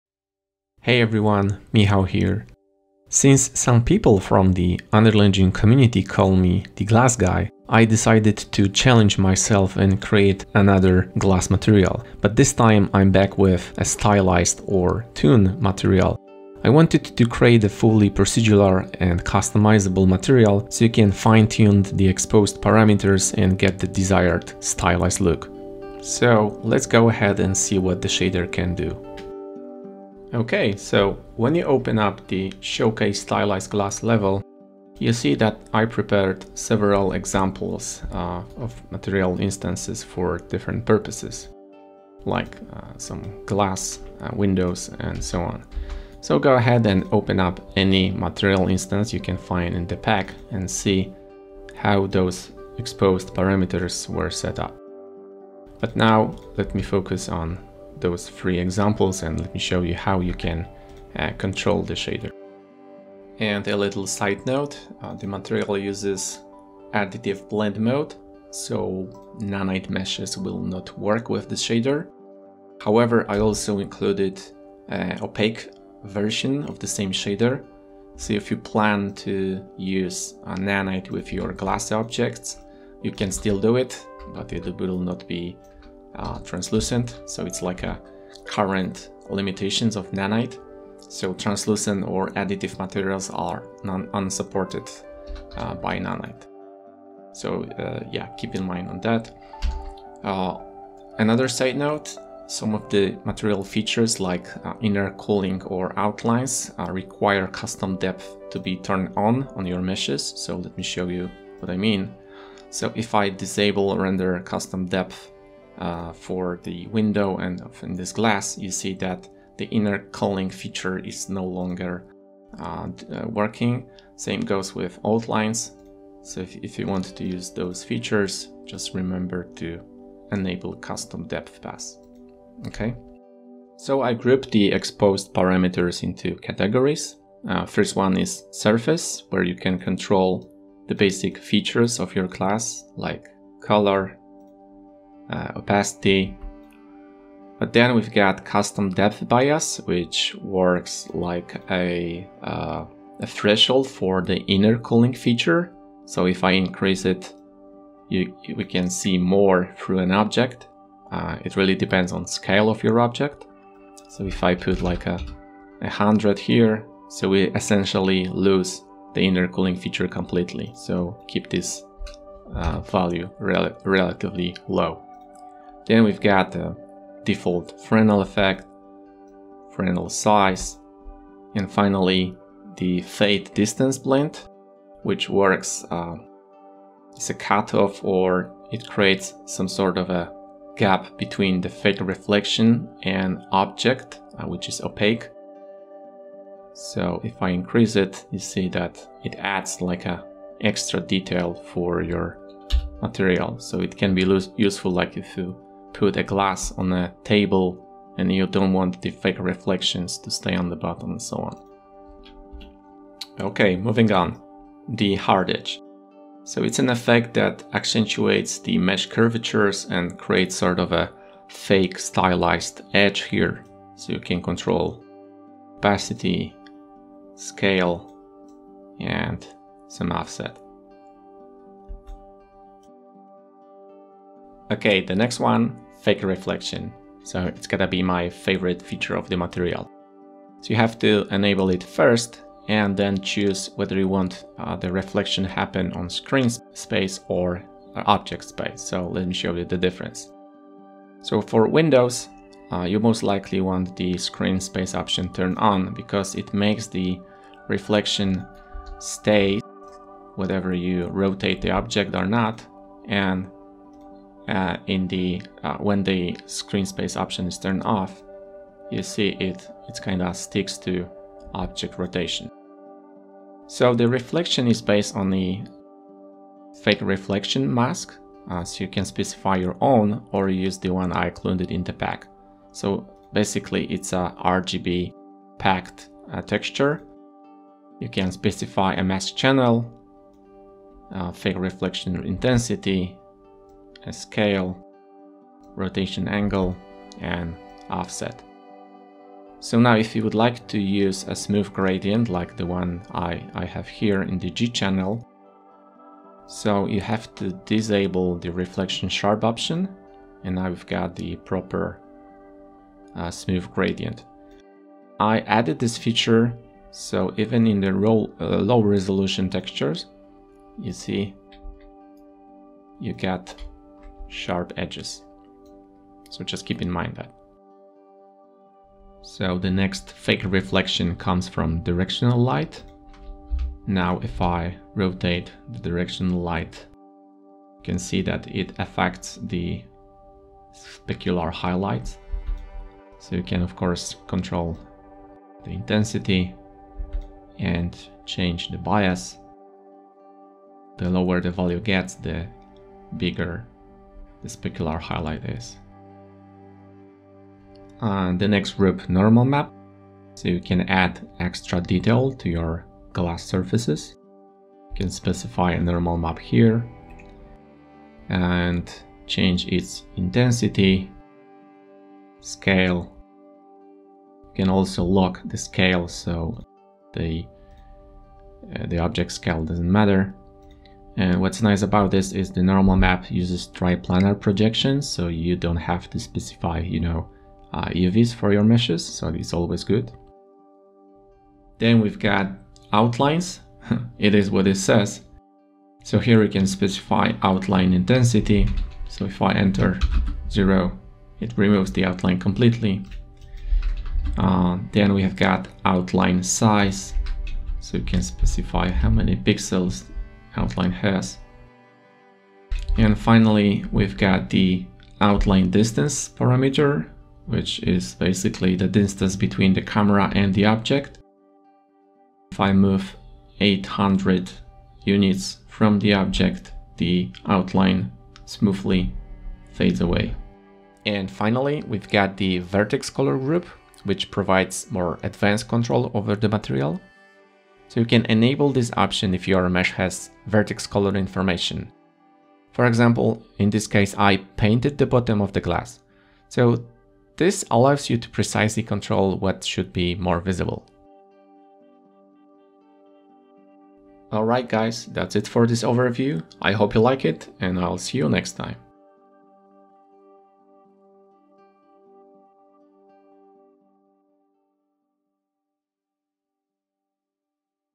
Stylized Glass Material for Unreal Engine 5 Overview
Stylized / Toon Glass Material Pack for Unreal Engine 5. A procedural and highly customizable stylized glass shader. Fine-tune the exposed parameters to achieve the desired stylized look of your glass. Drag-and-drop examples are included.

Timestamps:
00:00 - Intro
00:30 - Showcase
01:05 - Opaque Blend Mode
01:40 - Custom Depth Pass
02:12 - Surface Group
03:33 - Hard Edge Group
03:54 - Fake Reflection Group
05:28 - Fake Directional Light Group
05:57 - Normal Map Group
06:30 - Outlines Group
07:10 - Vertex Color Group
07:33 - Outro